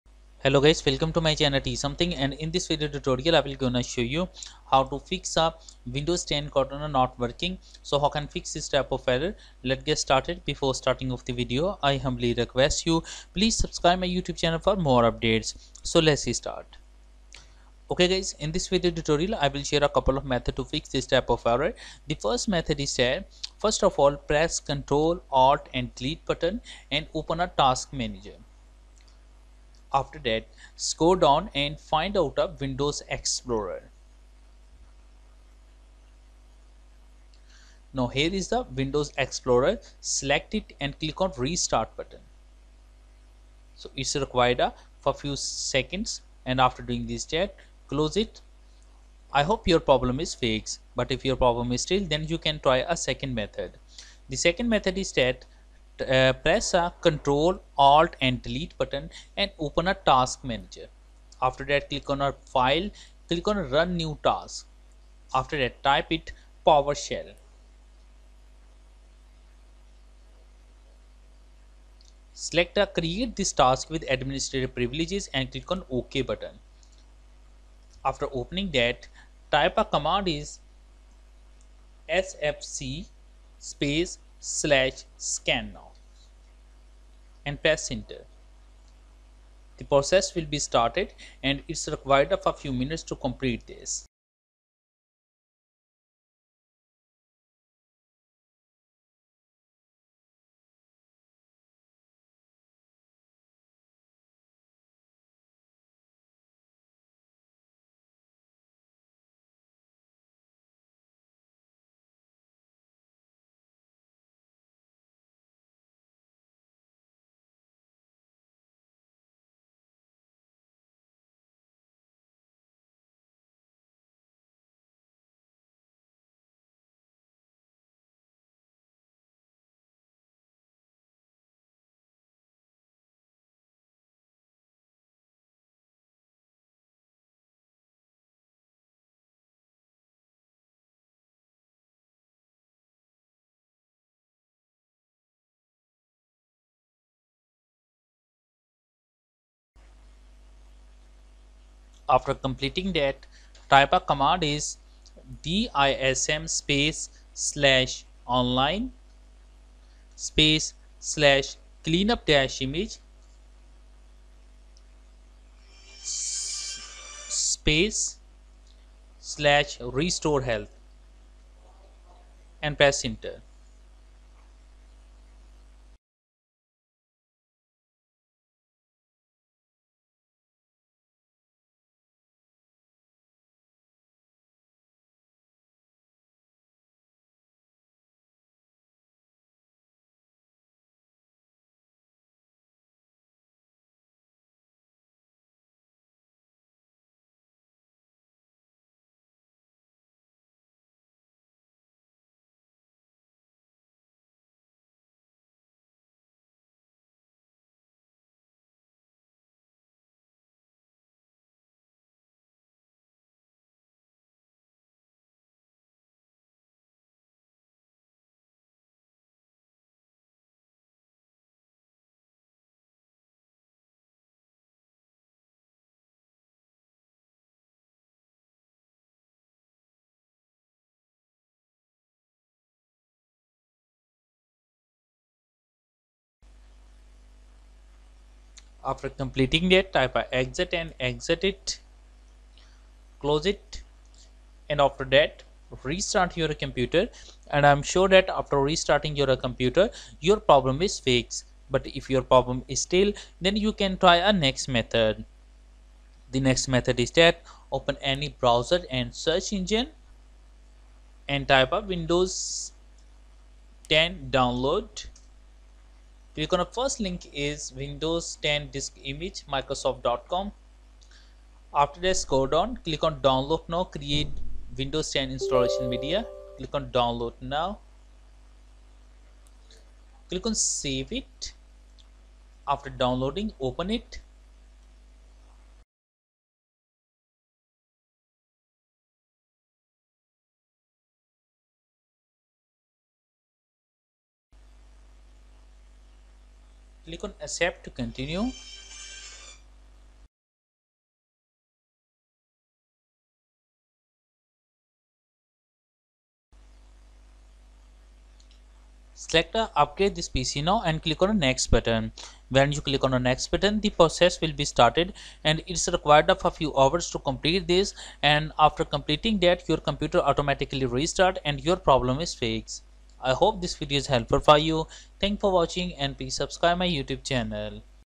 Fix Windows 10 Cortana Not Working
In this video tutorial i will gonna show you How to Fix Windows 10 Cortana Not Working.